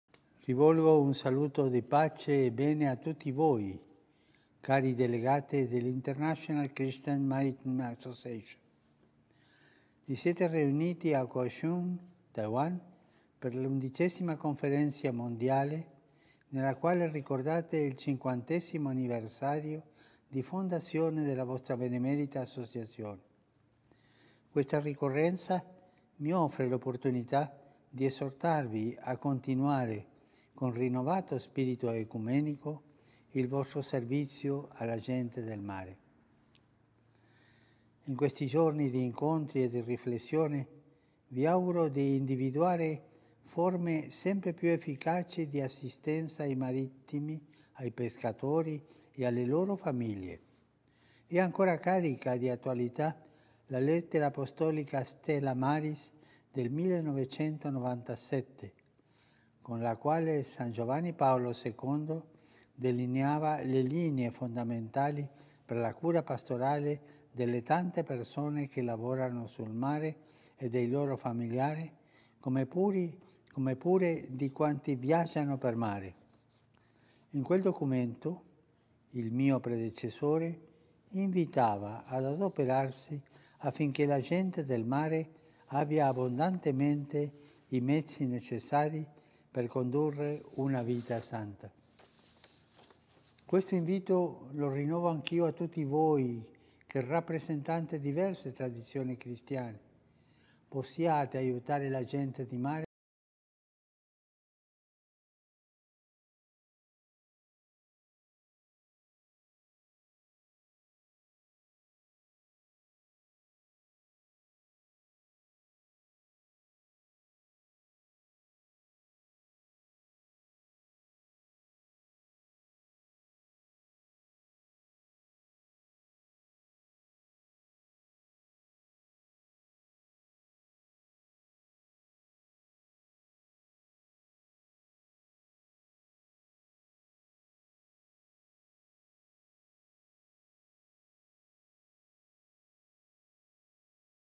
ICMA大會首度移師台灣 教宗親錄祝福影片完整公開(外交部提供)
國際基督徒海事協會50週年大會今年首次移師台灣舉行，教宗方濟各（Pope Francis）更特別錄製影片祝福、在21日大會上播放，完整影片今天曝光！教宗在片中表達對會議的期許，也呼籲與會教友依天主的教導，學習相互尊重，協助漁工及其他海事工作人員克服苦難。

影片原文翻譯：
I offer greetings of peace and goodness to all of you, dear Delegates of the International Christian Maritime Association.

You have gathered at Kaohsiung, Taiwan, for your Eleventh World Conference, during which you commemorate the fiftieth anniversary of the foundation of your praiseworthy Association. This anniversary allows me an opportunity to encourage you to persevere, with renewed ecumenical spirit, in your service to seafarers and maritime personnel.

In these days of encounter and reflection, I am confident that you will identify increasingly effective ways of assisting sailors, fishers and their families. In this regard, the 1997 Apostolic Letter Stella Maris remains most timely. There, my Predecessor Saint John Paul II set forth basic principles for the pastoral care of seafarers, their families and all those who travel by sea, and urged that every effort be made to see that they are “provided abundantly with whatever is required to lead holy lives” (II § 2).

I renew that invitation to all of you who represent different Christian traditions. May you help seafarers and maritime personnel to know Jesus Christ and to live in accordance with his teachings, in respect and in mutual acceptance.

I encourage you to surmount whatever difficulties you may encounter in your mission and to promote with conviction the spirit of ecumenism. I accompany you with my prayers and my blessing, which I readily invoke upon you, upon your deliberations in these days, and upon all those entrusted to your pastoral service.